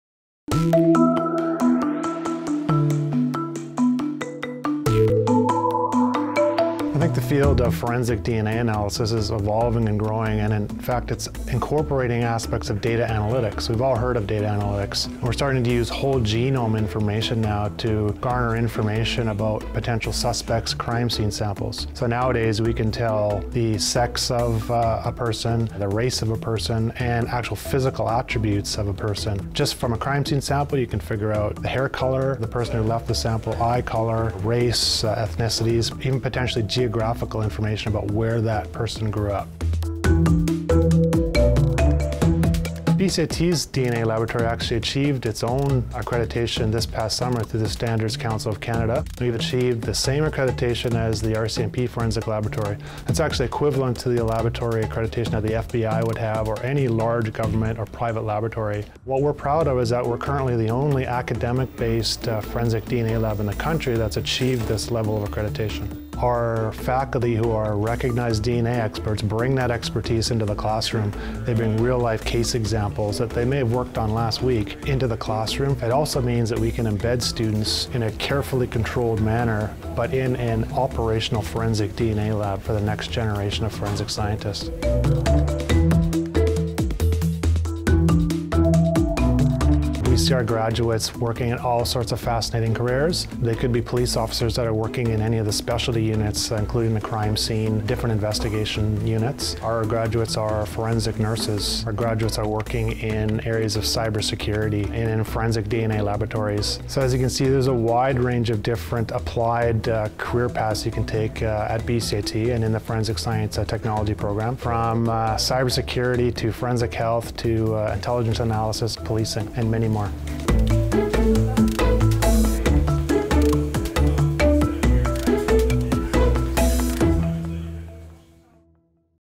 BCIT Forensics — Job opportunities in Forensic Science and more
BCIT's Forensic Science and Technology program is the most comprehensive of its kind in Canada. offering course and programs like forensic science, forensic health science, crime and intelligence analysis, digital forensics, fraud and financial crime investigation, and cyber security. Find out what makes BCIT's Forensic DNA lab so unique and what kinds of job opportunities are available for BCIT grads in Forensic Science.